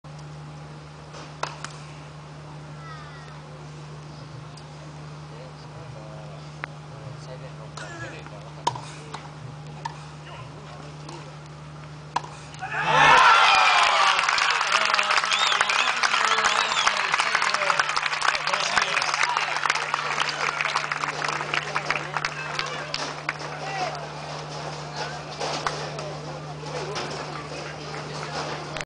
Zeballos/Prodon acceden a semifinales de la Copa Claro 2012.
El tenista argentino Horacio Zeballos y el francés Eric Prodon vencen a los sembrados número 1, los italianos Potito Starace y Daniele Bracciali, por 1-6, 6-3 y 12-10. De esta manera, logran el pase a las semifinales de la Copa Claro 2012 en dobles.